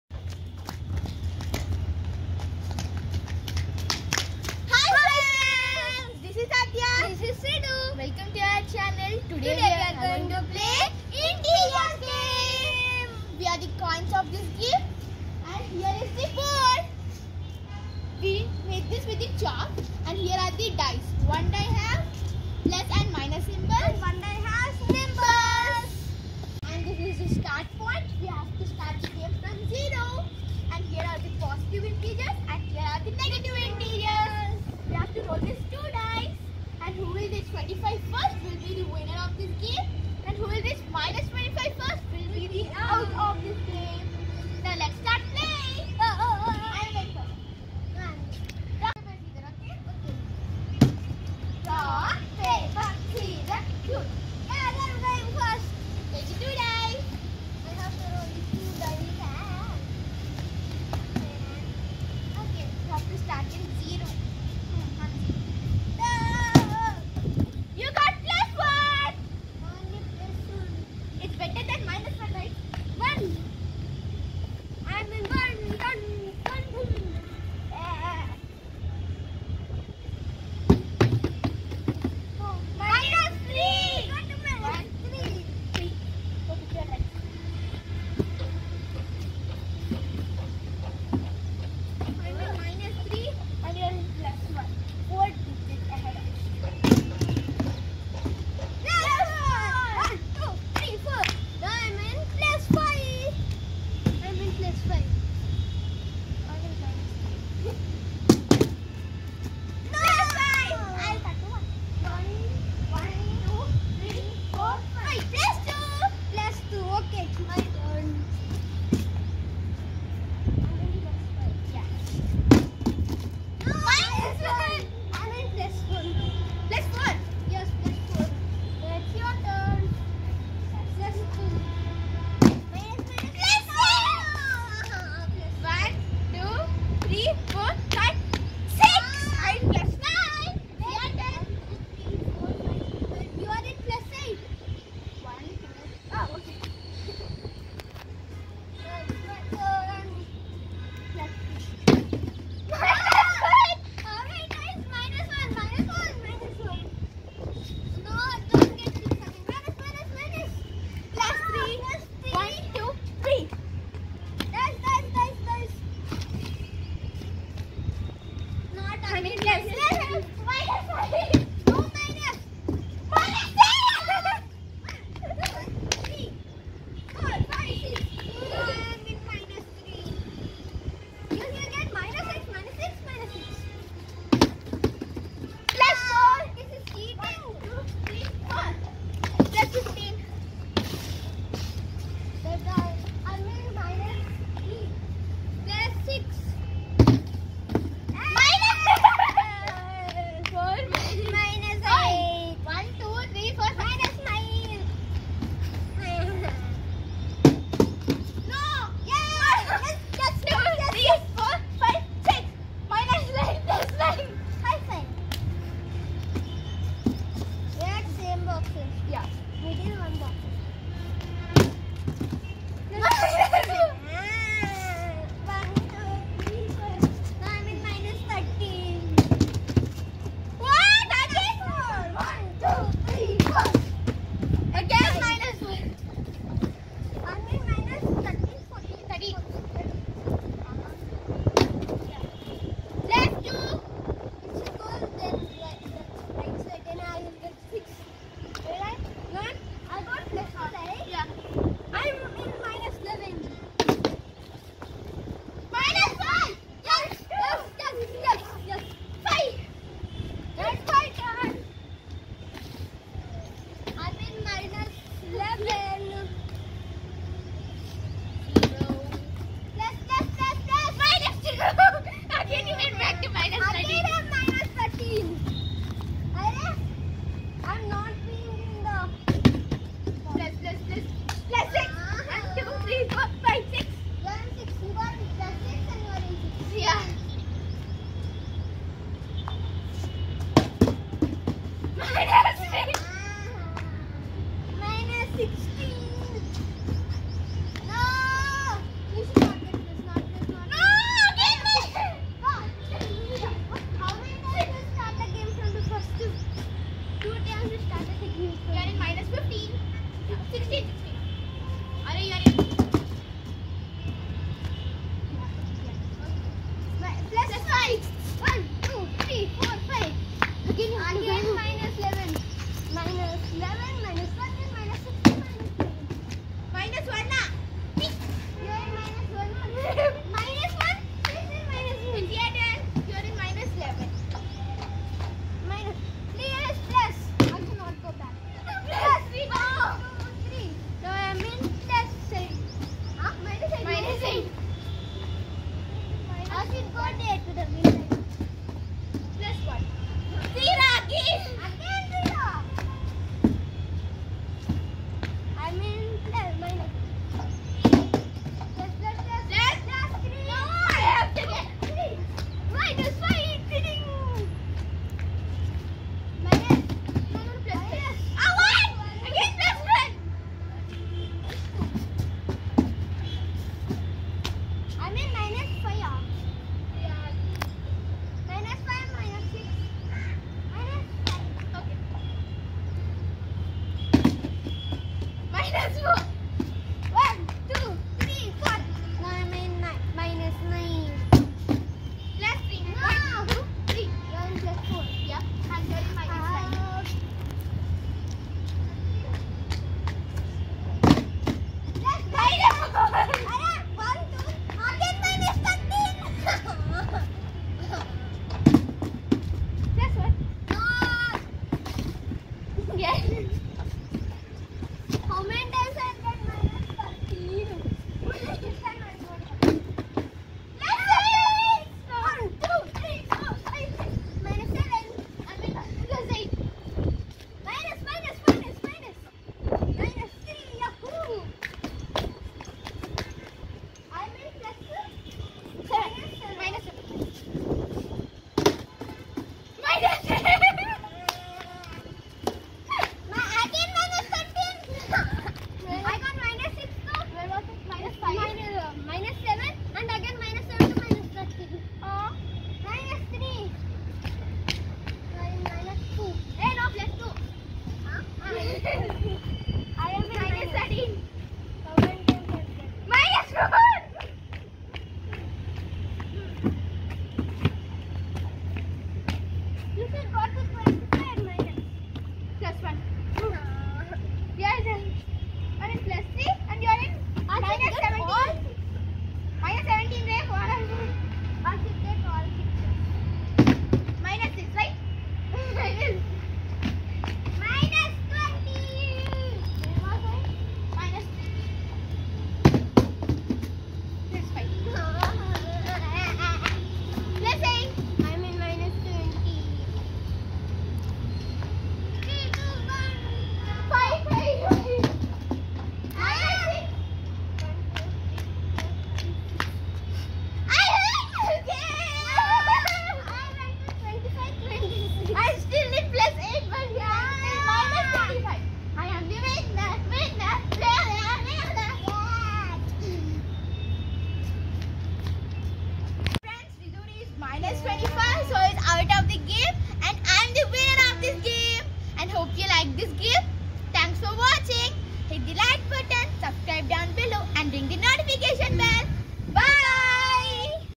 Fun game with maths|| Integers game|| Aadhya and Hridhu
Integers board game which we can prepare  ourselves easily and play the game. Just need to mark negative numbers, zero and positive numbers and need two dice to roll. Here in this game start the game from zero and the one who reaches the no 25 first is the winner of the game or the one who reaches -25 first is the loser of the game.